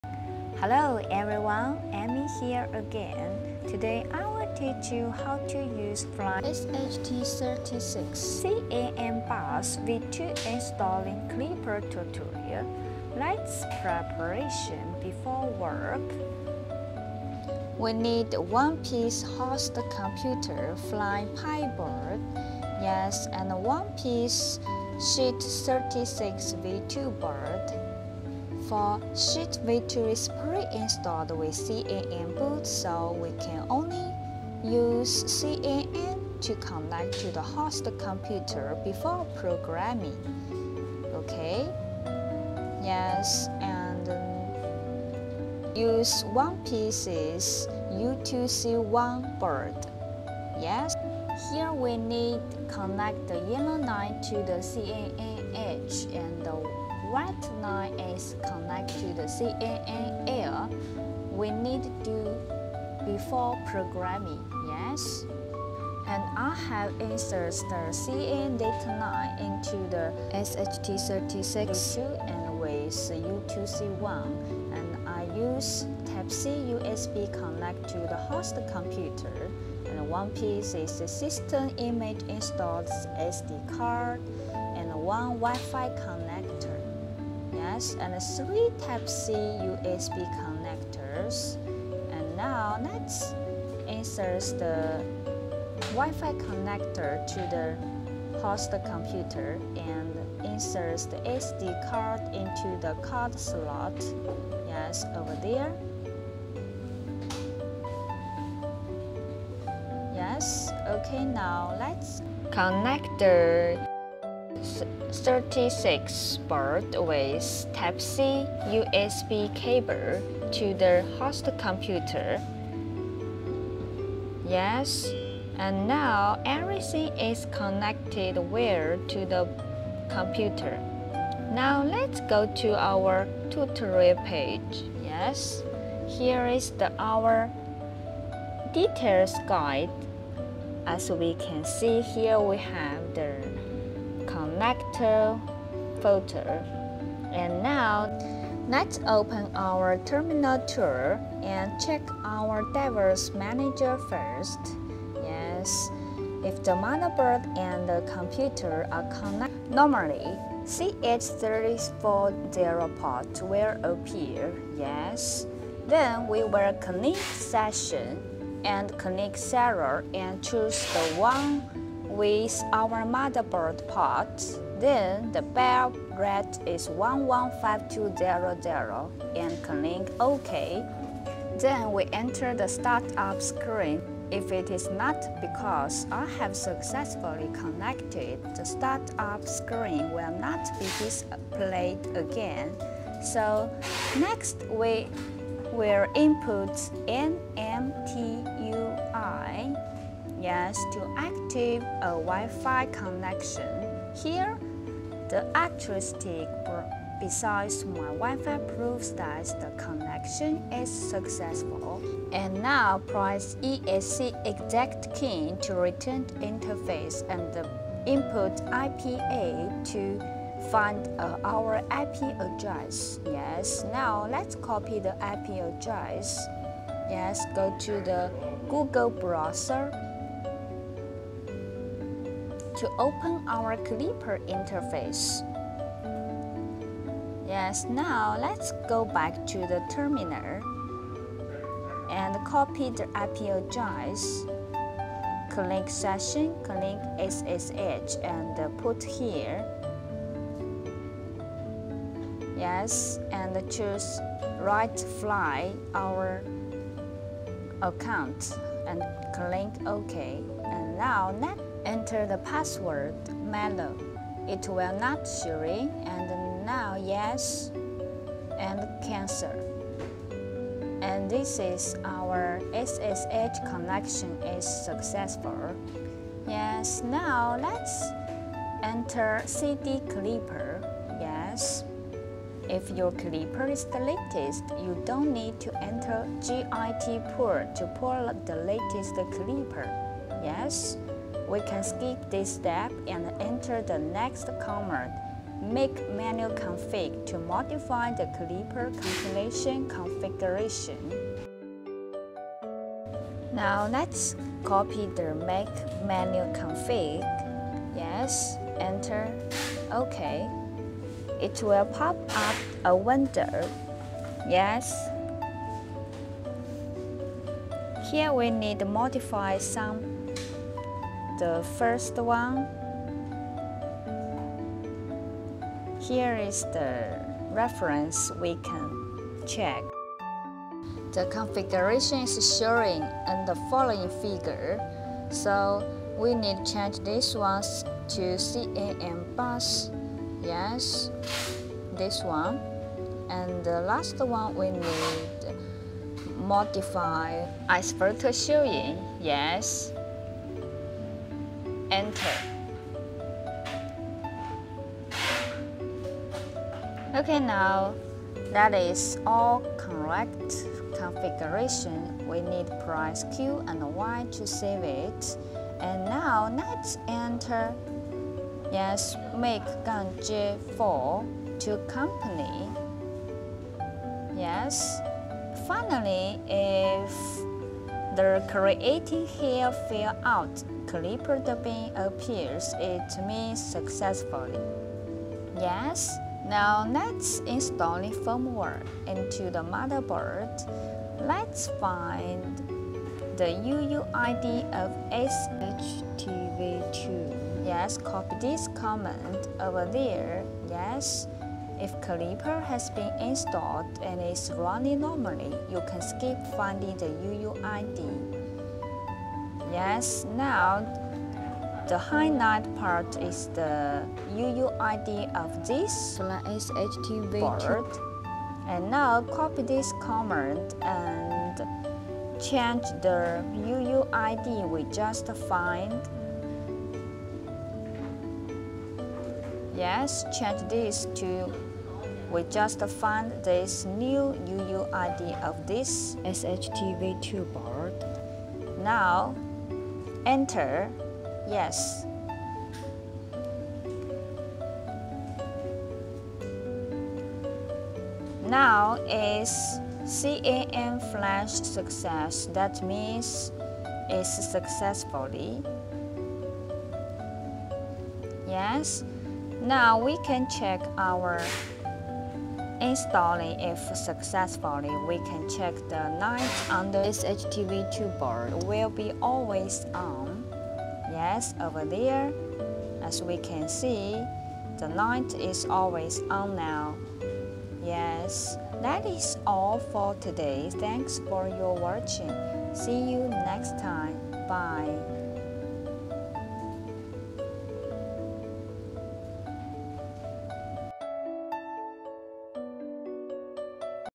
Guide: Mellow Fly-SHT V2.0 Board install klipper - step by step guide for 3d printer beginners
This video will show you how to use Mellow Fly-SHT V2.0 Board install klipper -  step by step guide for 3d printing beginner